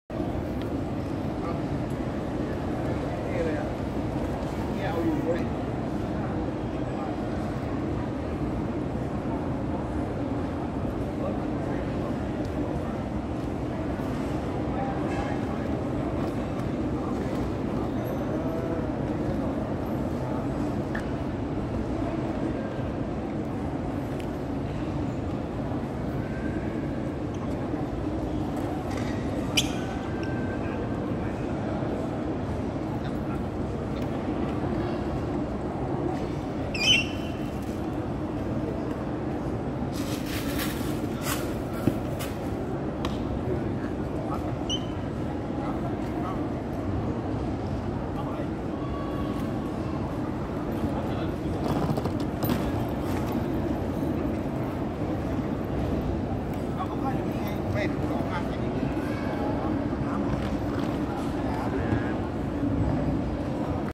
Tailandêses em Guarulhos
Espremendo na mala caixas de bombom garoto e paçocas